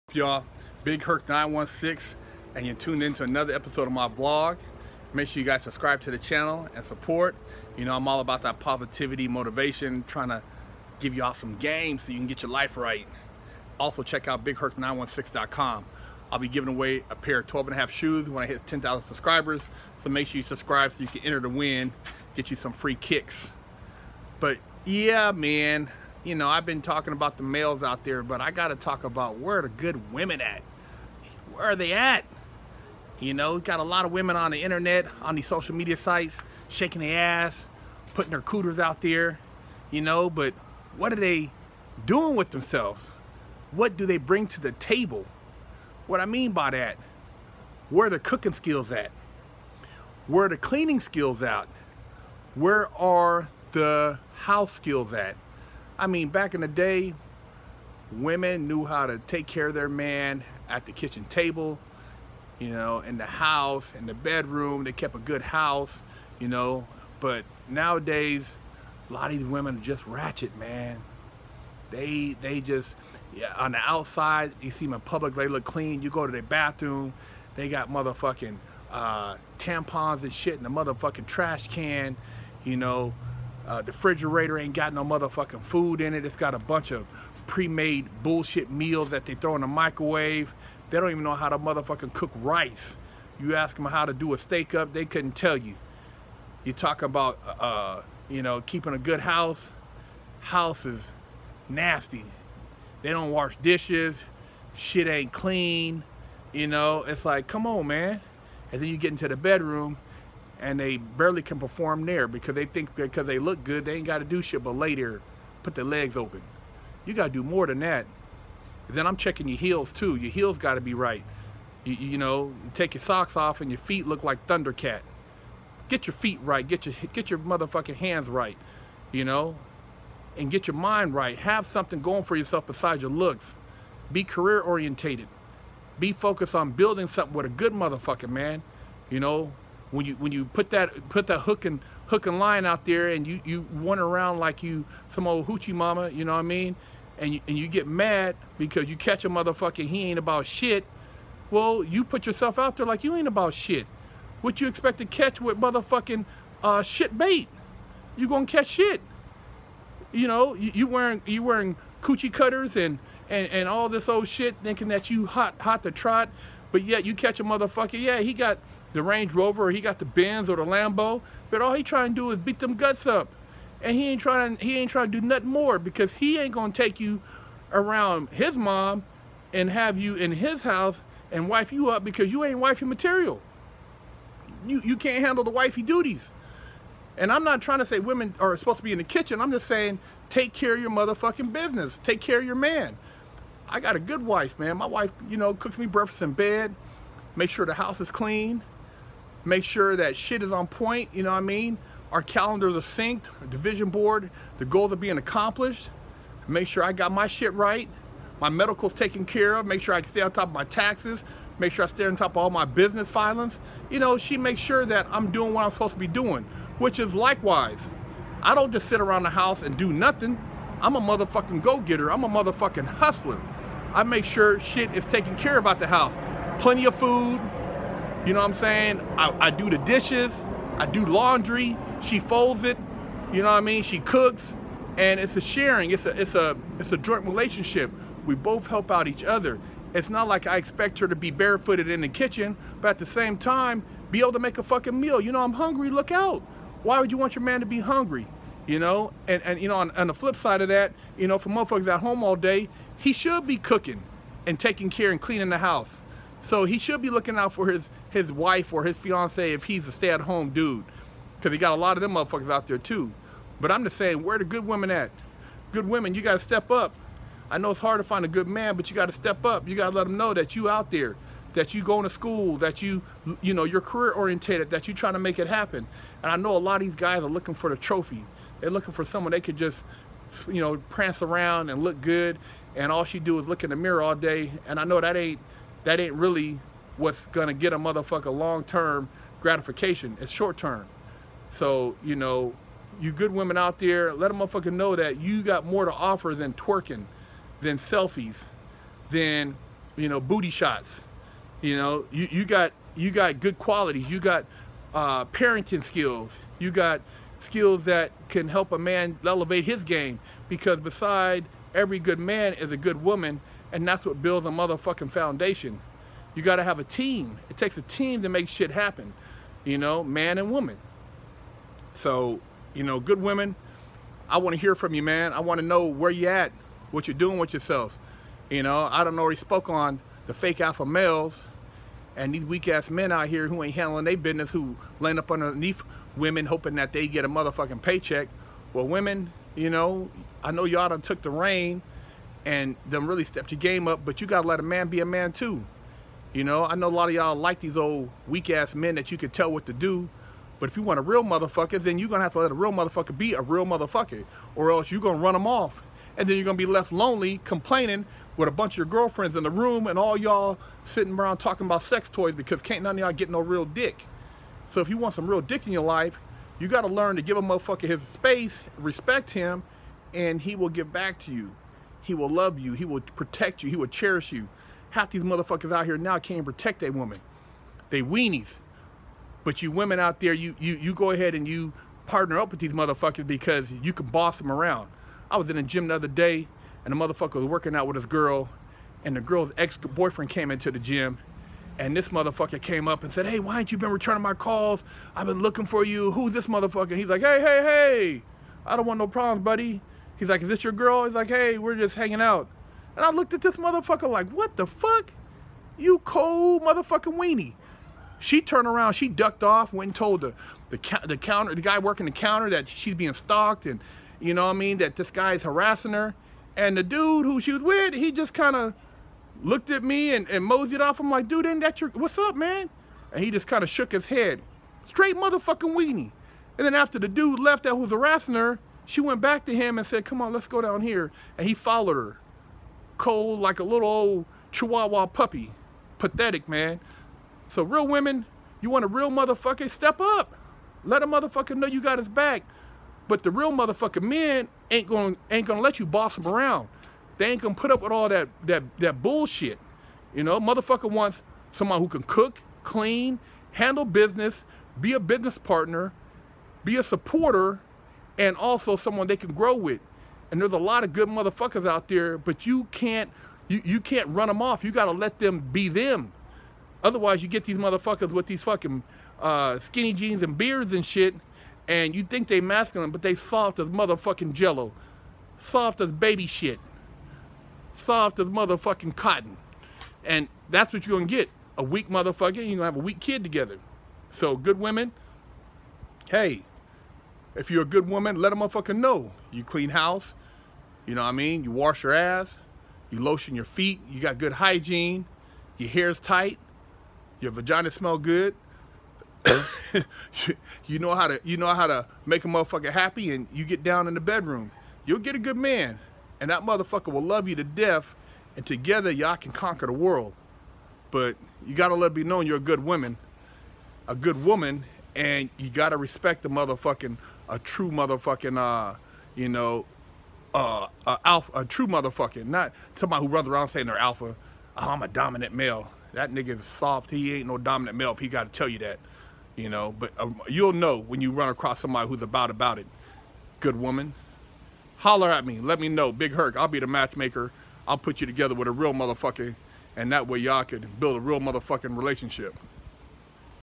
Where Are The Real Women
I’ve spoken about men and how they have dropped the ball when it comes to playing their role in society, but women have also come up short. Where are the real women. Step up and represent.